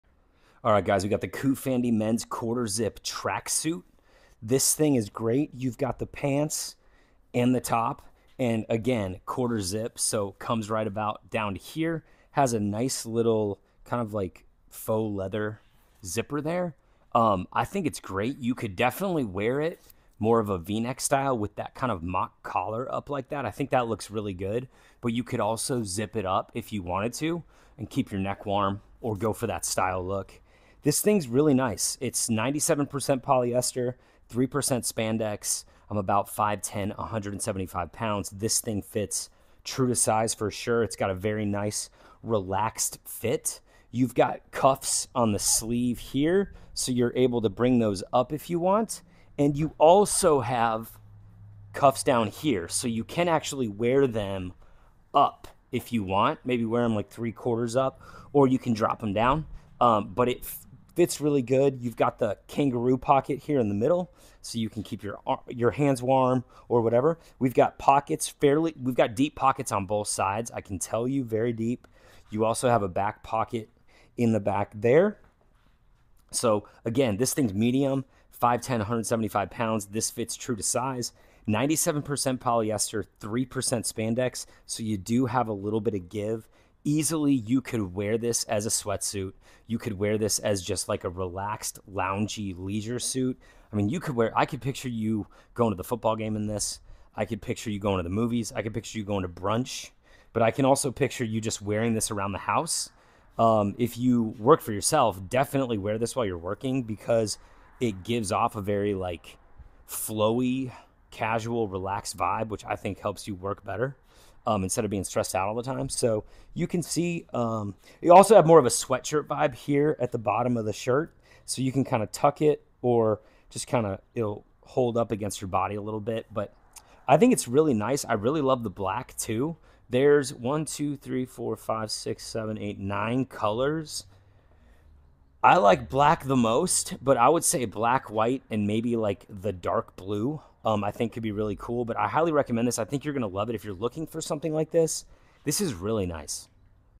COOFANDY Men's Tracksuit 2 Piece Quarter Zip Sweatsuit Sets Casual Athletic Running Jogging Suits
Quality Fabric: Men's 2 piece tracksuit set is made from soft polyester and spandex fabric, lightweight, breathable and durable. Men's sweatsuits 2 piece sets will bring a flexible and comfortable wearing experience.
2 Piece Tracksuits for Men: Men's sweatshirt is designed with 1/4 zipper, a kangaroo pocket, ribbed cuffs and hem. Elastic waist and adjustbale drawstring men's jogging sweatpants is crafted with 2 side pockets and a back pocket. Easy for you to carry keys, phone and other small items etc.
Matching With: Matching men's sweatsuits set with a sunglasses for a stylish street look. Pairing men's quarter zip tracksuits with a t-shirt and sneakers for a sporty casual look.
Occasions: Men's jogging suits set is suitable for casual, daily wear, gym, workout, sports, running, walking, travel, hip-hop and other outdoor activities. Men's two-piece casual tracksuit is a great Halloween and Christmas gift for yourself, your friends, husband, boyfriend, son, dad, etc.
Garment Care: Machine Washable. Men's tracksuit set 2 piece jogging outfits are standard US size.Please refer to our size chart to ensure a perfect fit.